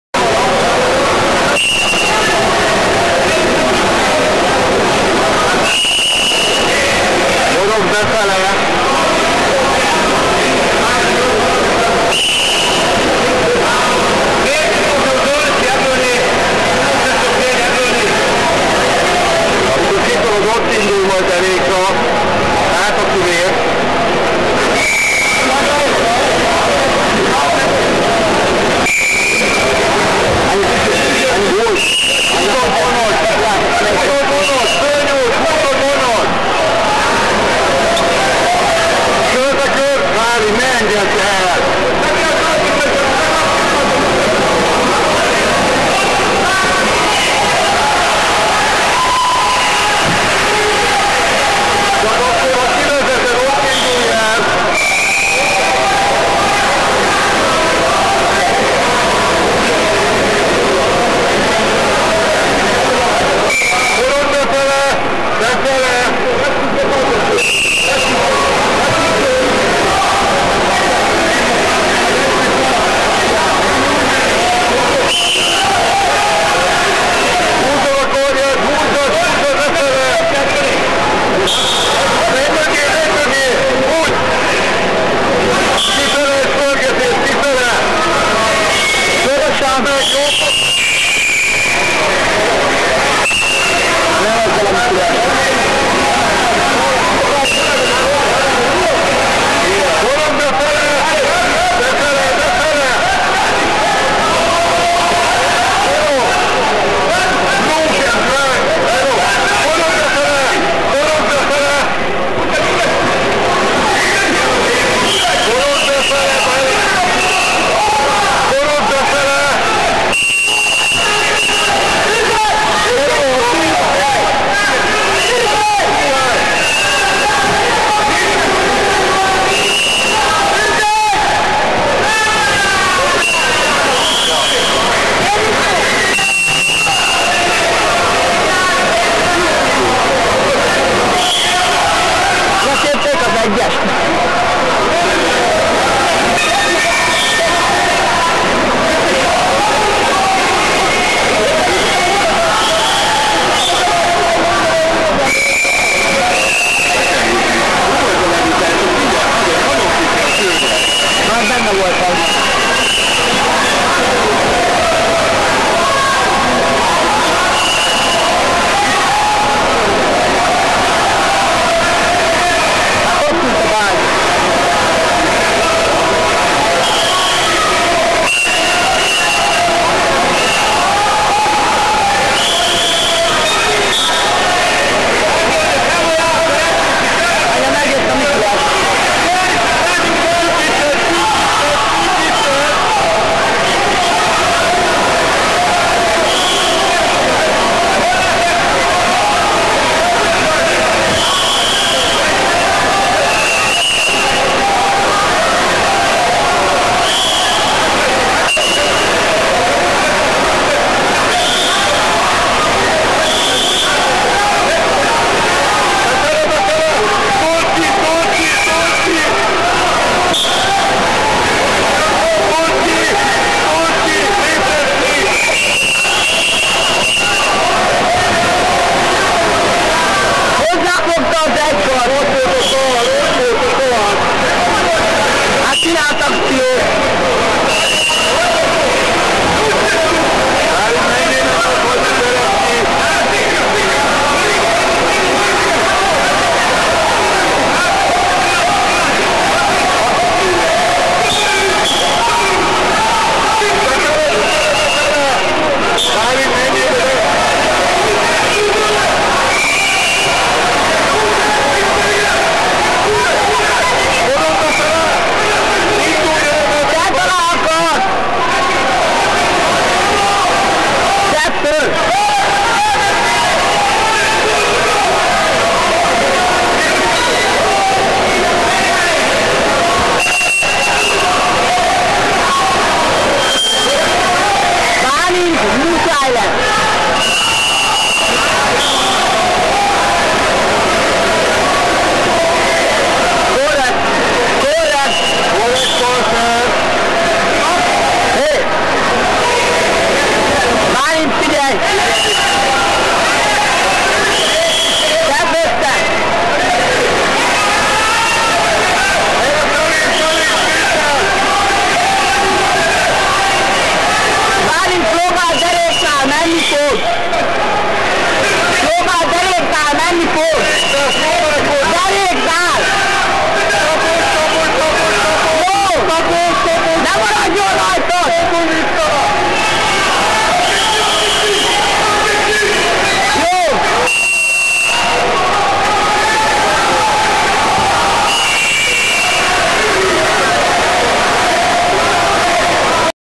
Zágráb / Diák 53 kg Agron Sadikaj - Balint Vátzi
Zágráb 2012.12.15. Karácsonyi Kupa nemzetközi kf. birkózó verseny.